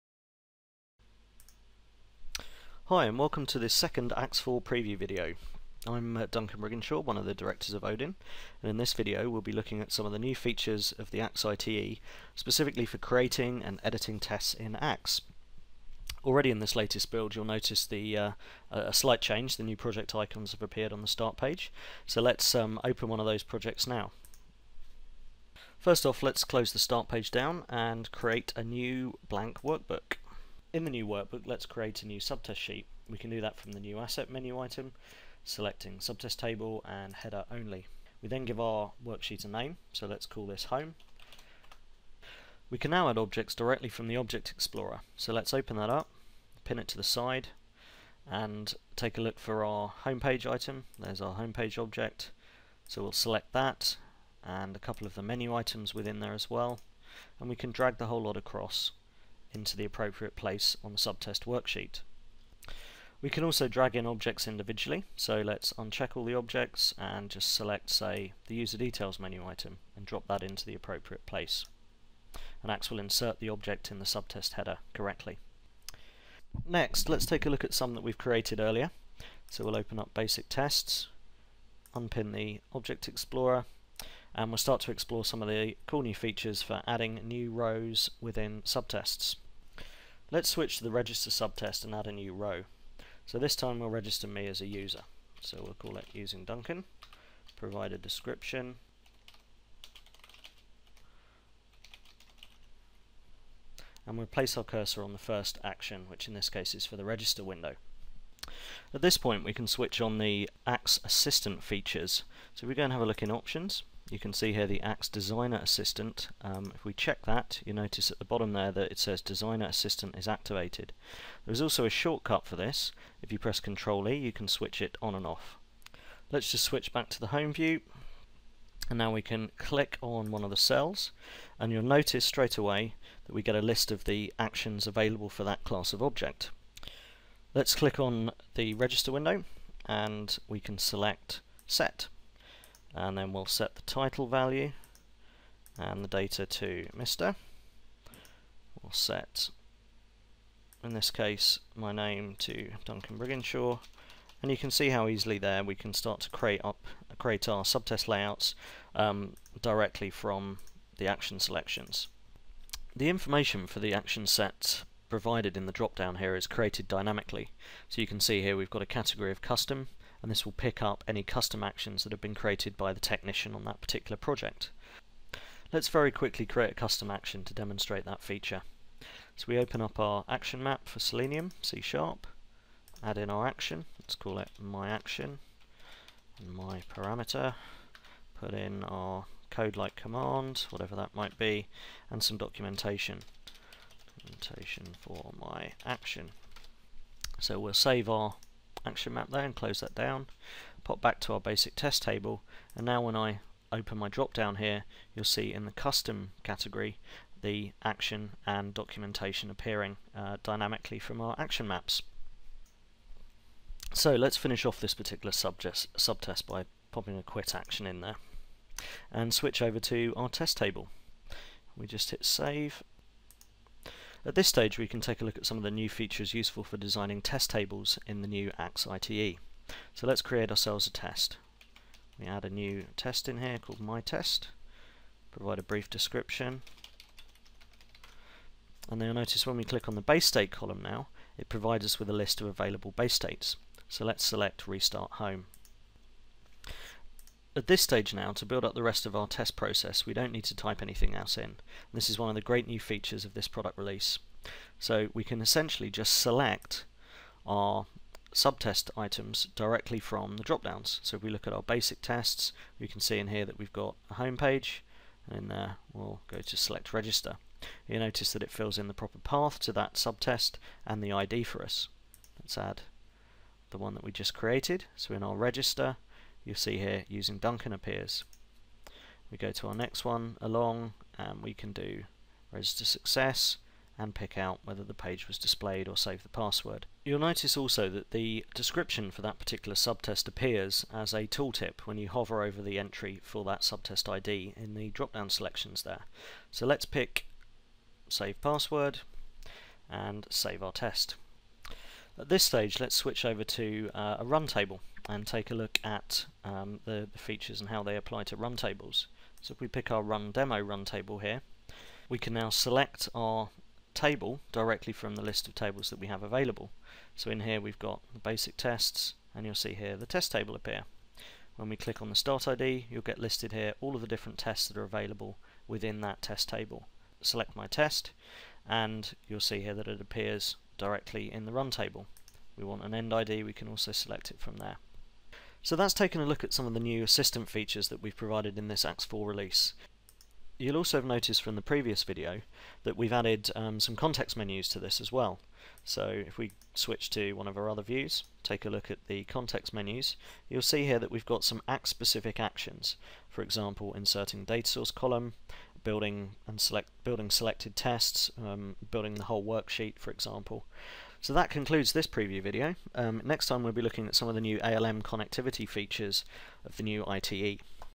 Axe v4.0 - Preview Video 2 - Test Design Assistant Features
In this second Axe v4.0 preview video, Duncan Brigginshaw from Odin demonstrates some of the new test design assistant features in the all-new Axe ITE.